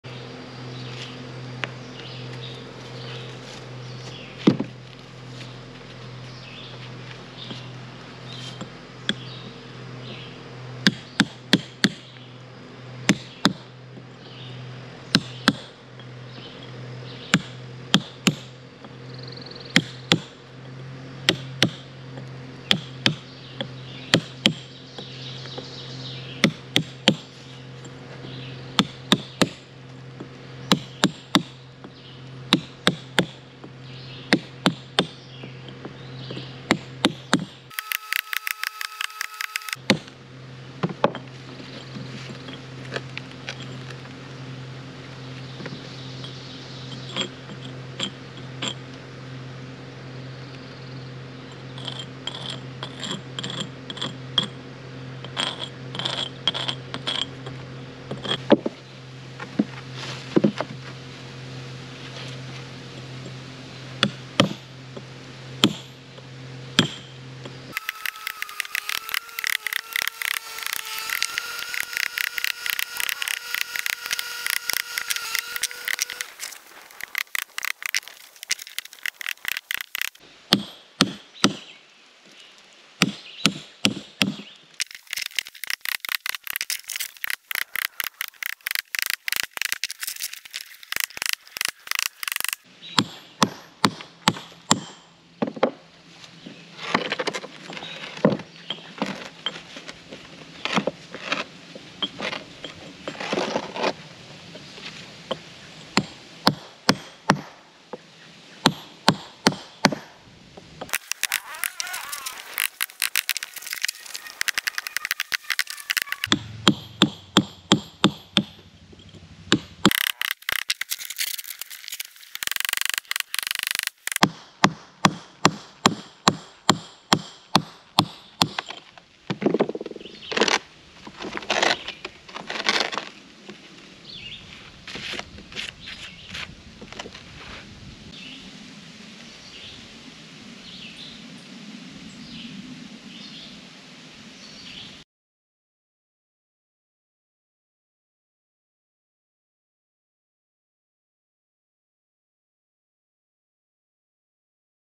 Building a BarnBQ Part 2: First Stone Split
Trying to split the first stone for the BarnBQ cladding